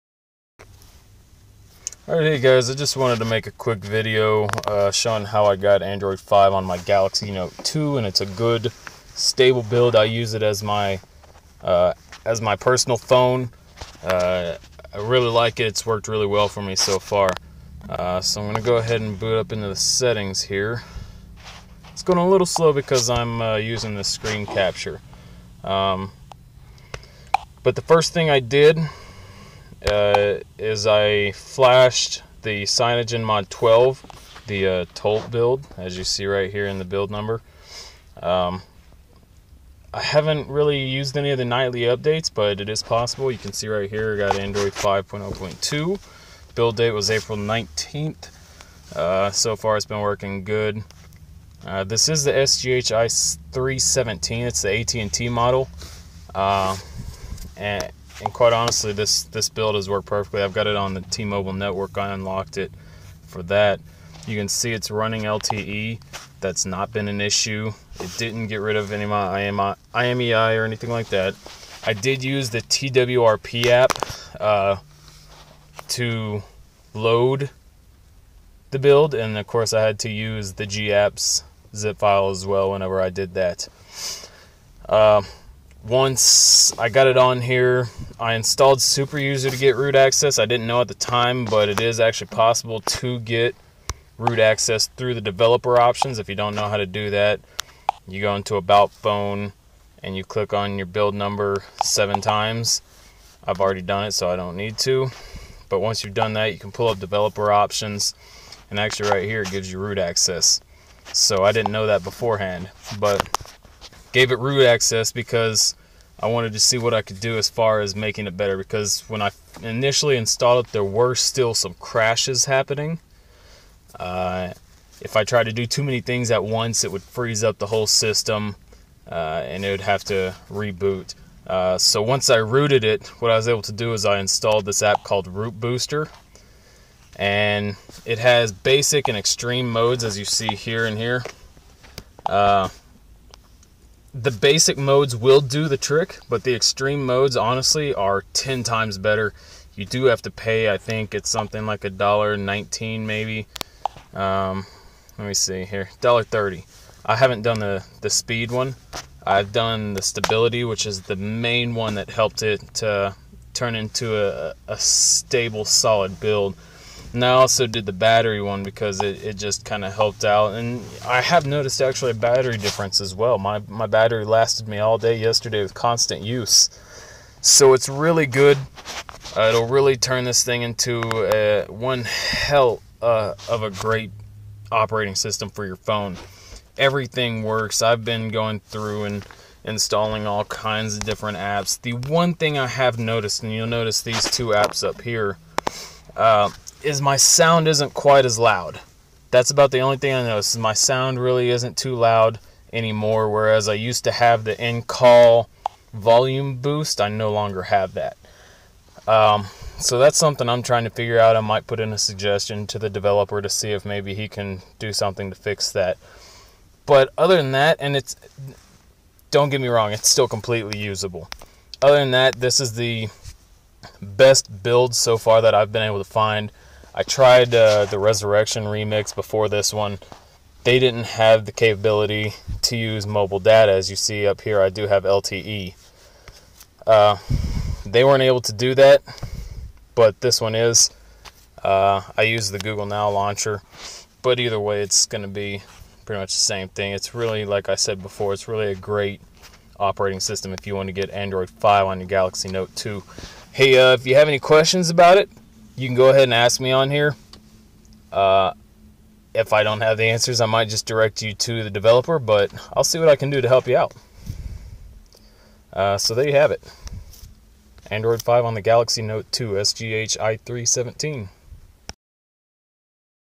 Cyanogenmod 12 Lollipop on Galaxy Note 2
Using the t0lte build of CM12 I installed a stable build onto my sgh-i317.  If there is any interest I will make a how to instructional video on installing it and everything I did to make it stable.